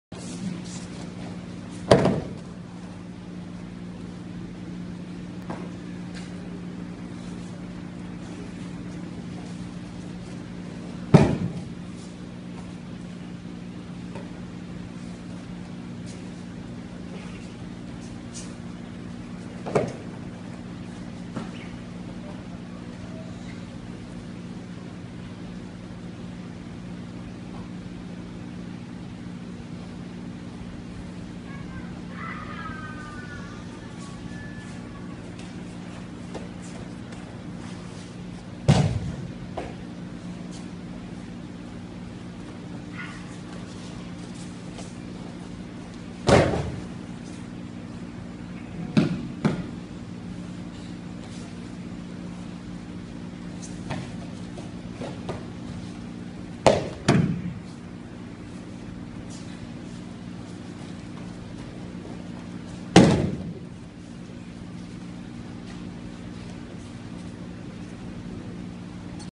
Koji Hasegawa (6th Dan Aikikai Aikido)
Demonstration at Penang, Malaysia. 23rd October 2016. Student of Kunio Yoshimoto Shihan (7th Dan Aikikai Aikido) from Aikido Shoshinkai Japan.